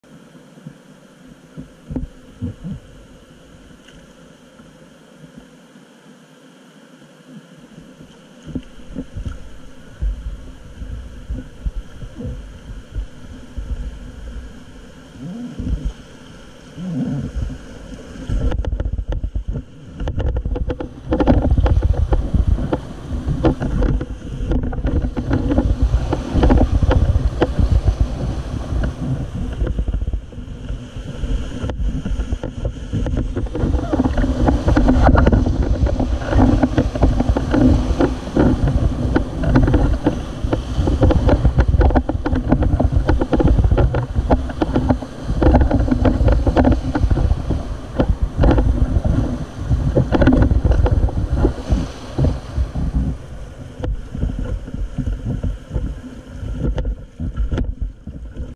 Stonycreek River Shade Creek Surf Wave Rapid - R1 whitewater rafting
Stonycreek river Johnstown Pa Feb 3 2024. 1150cfs 4.2 ft. aire tributary 9.5 HD, NRS PTC 60", tough river stuff thwart bag,  Rtic sling cooler, level six dry suit, level six compact throw bag and belt, nrs ion pfd,  and wrsi current helmet.